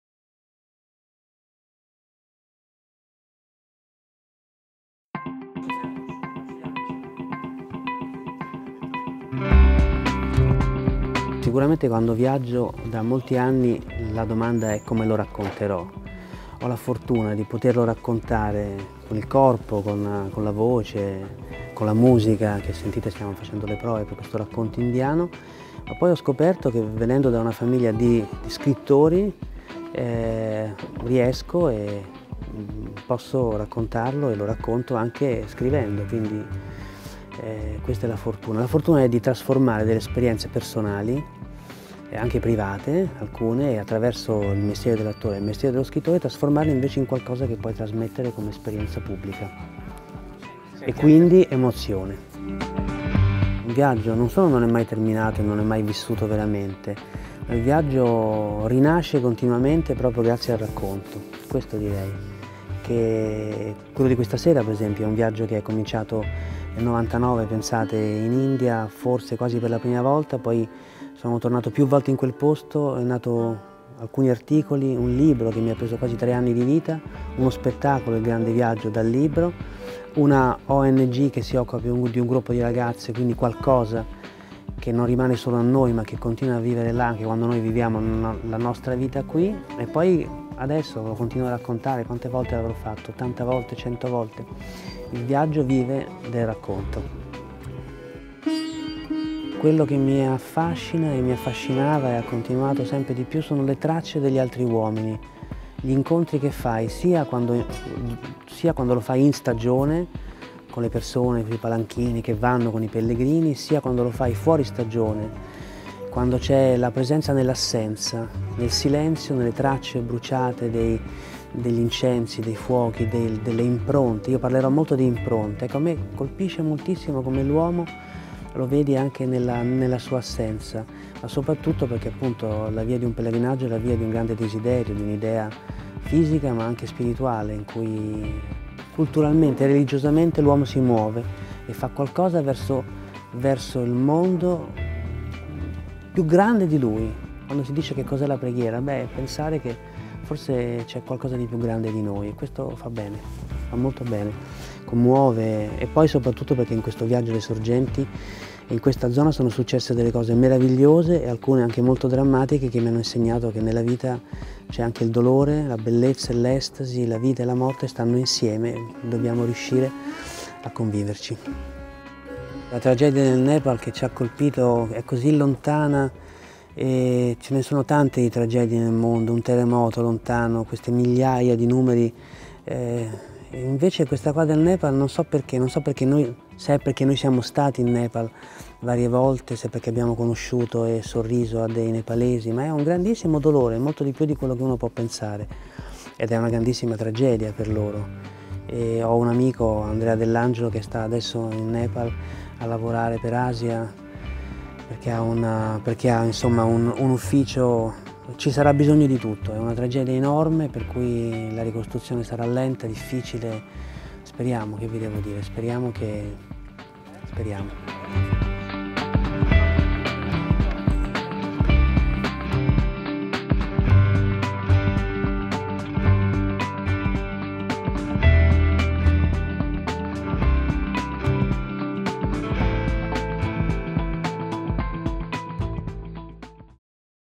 Giuseppe Cederna racconta le mille anime dell'India
Intervista a Giuseppe Cederna ospite del 63° Trento Film Festival dove ha presentato lo spettacolo teatrale "Le mille anime dell'India".
Andrea Bianchi lo ha intervistato durante le prove, al Teatro Sociale di Trento.

Riprese e Montaggio: Andrea Monticelli